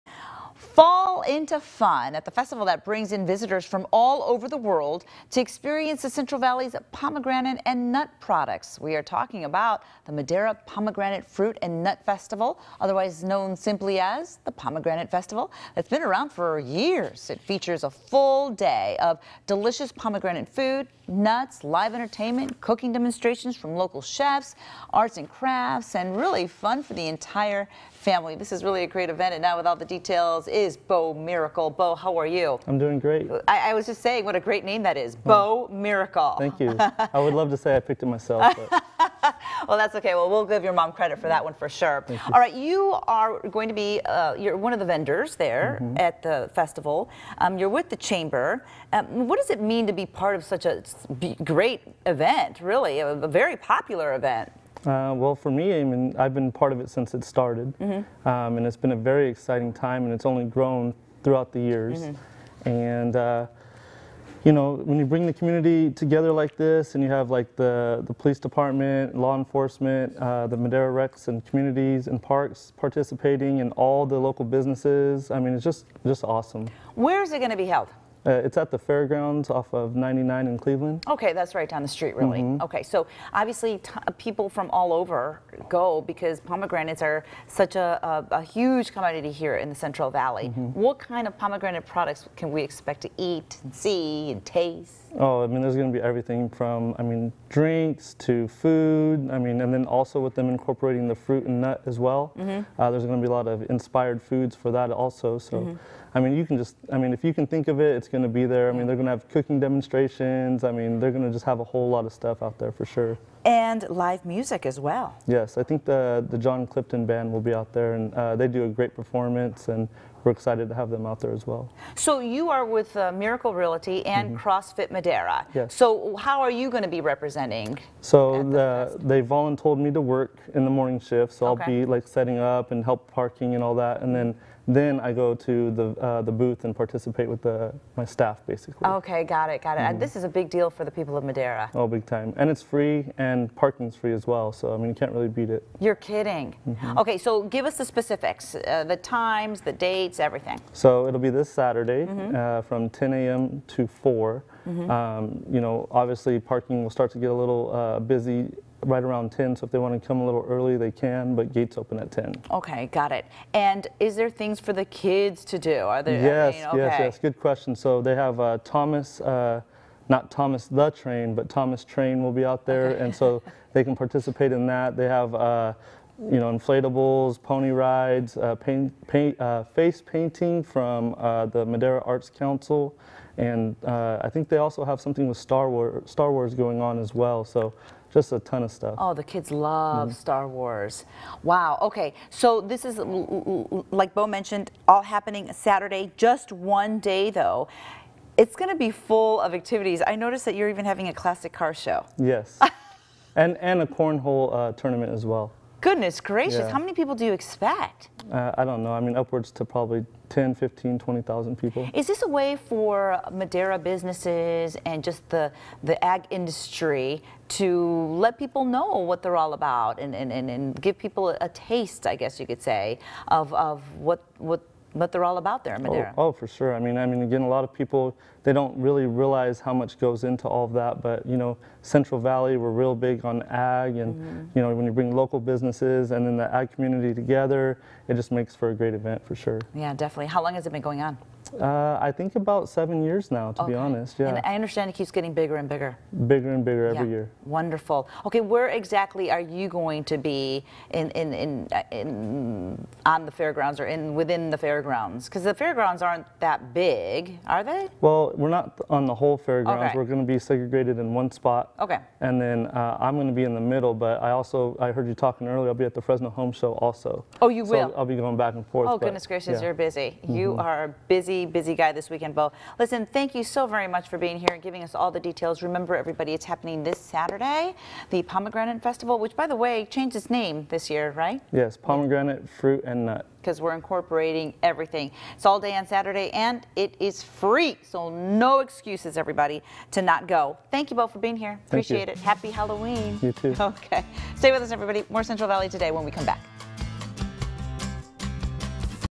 Central Valley Today
Celebrate everything about the delicious pomegranate at The Madera Pomegranate, Fruit and Nut Festival this Saturday.